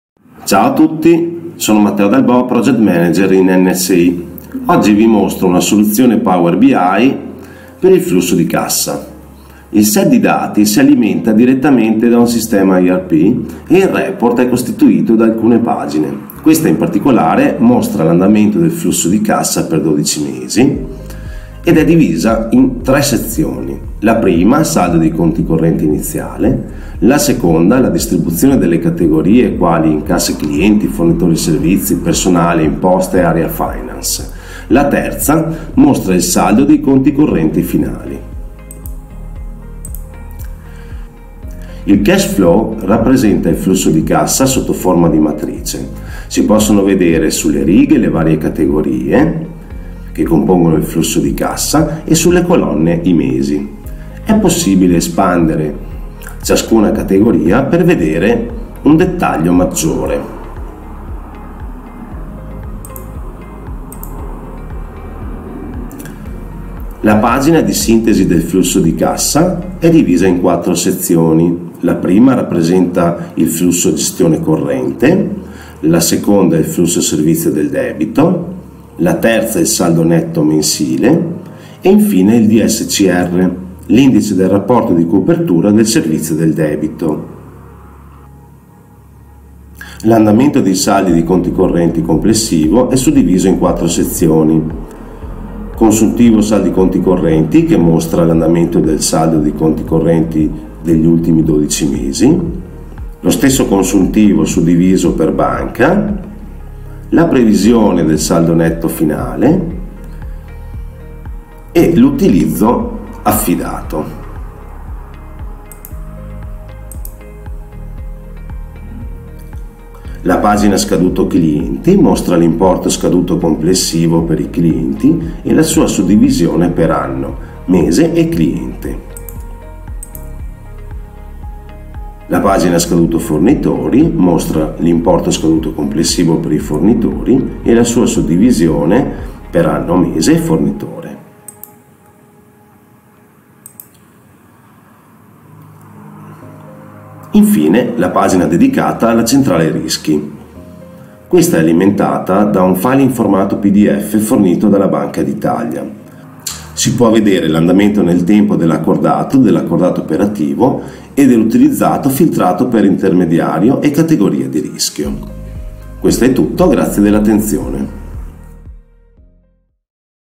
Come funziona Microsoft Power BI per il Cash Flow | Video Tutorial
Sai come utilizzare Microsoft Power BI per monitorare il flusso di cassa della tua azienda?
Il nostro Project Manager Matteo ti spiega come fare.